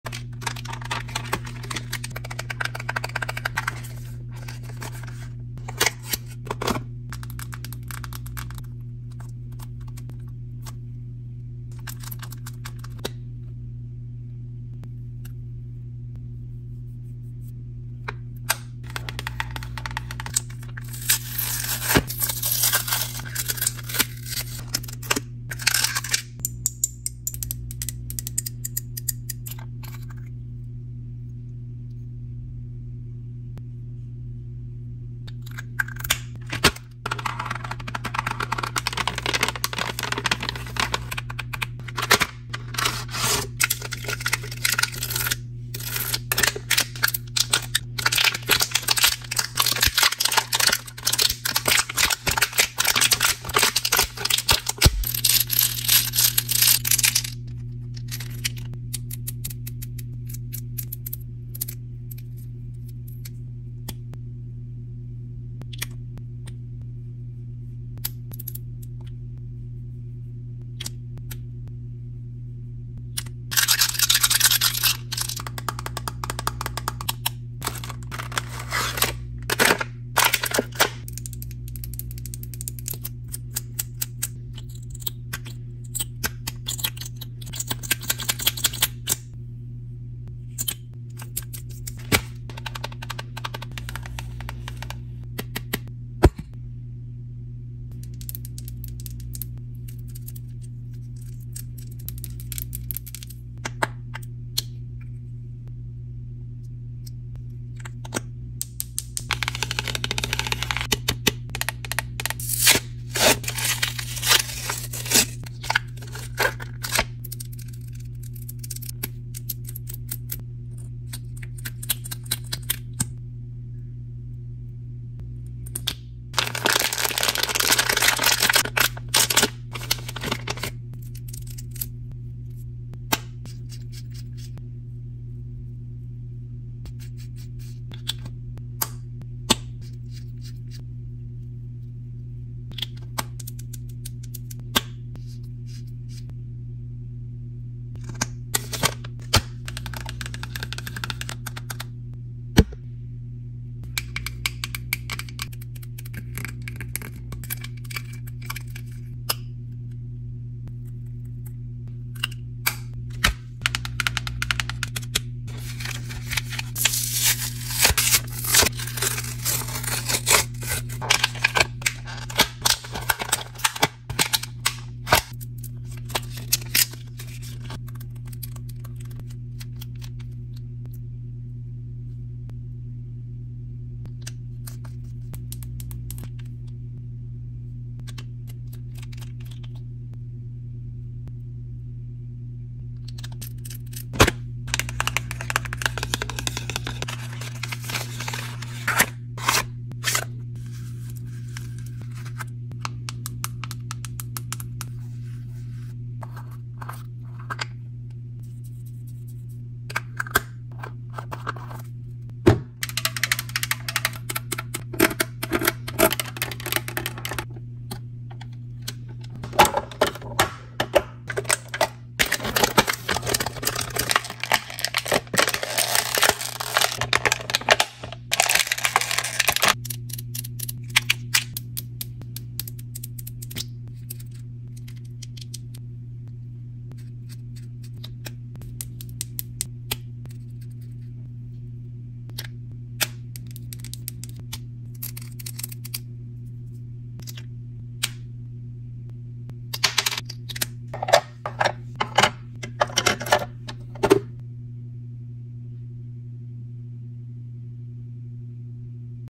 Cute TEMU Lip Balm & Mini Lipstick Haul (part 2)Unboxing Satisfying Video ASMR! asmr asmrmakeup 💄
Let's Open these Cute Mini Lipstics and Lip Balms From TEMU!